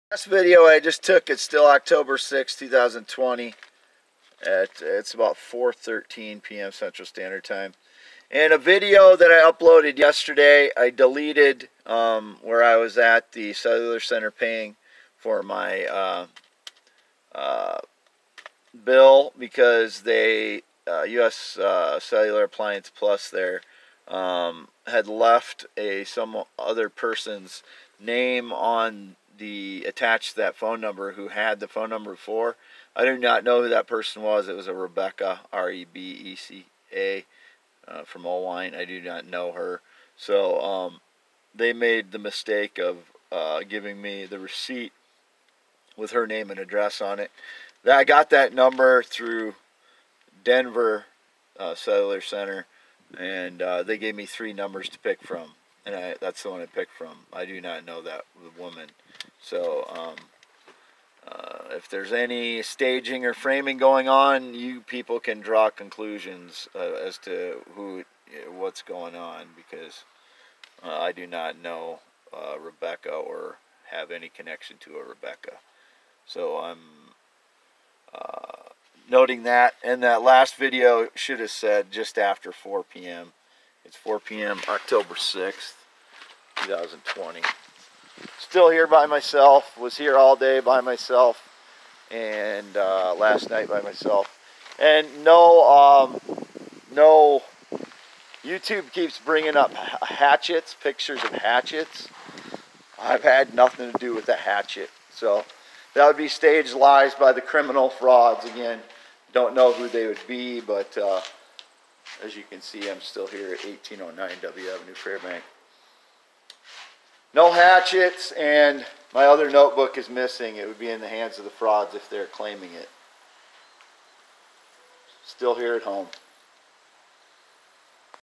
Video 2 today. Youtube keeps prompting pictures of hatchets. No incident with any hatchet.
I do not know who "Mr Beast" on Youtube is. Youtube keeps prompting his picture as well as pictures of hatchets.  Any incident involving a hatchet is a staged lie by those who trespass and break and enter thieving me.  Also dont know who has my other notebook with many dates Ive posted.  Would be a criminal fraud if pretending to be me.